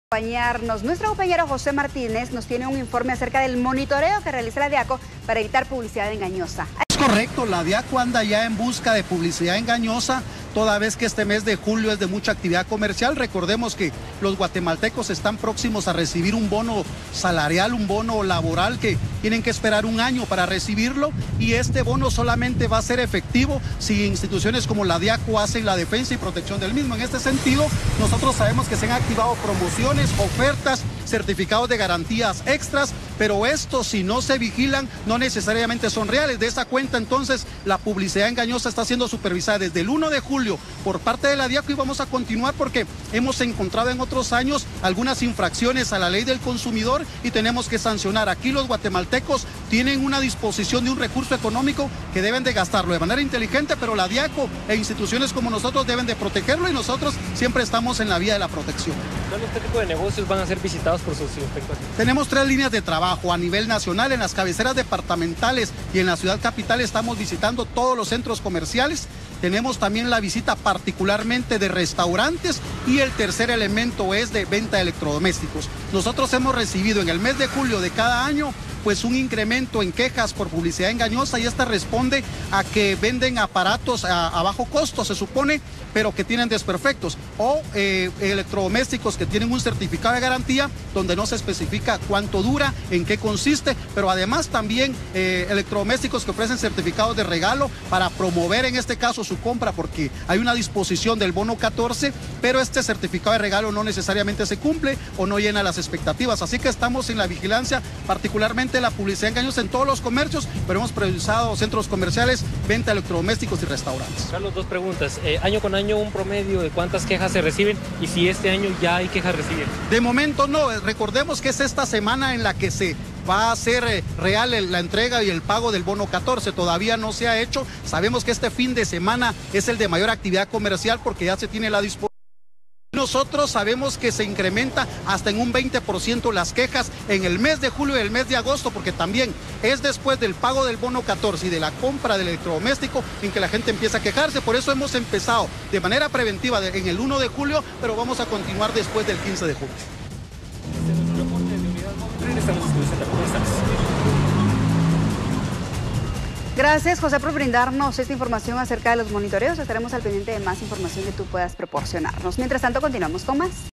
Diaco realiza monitoreo en comercios
La Dirección de Atención y Asistencia al Consumidor (Diaco) realiza un monitoreo por la capital para evitar publicidad engañosa. Buscan evitar publicidad engañosa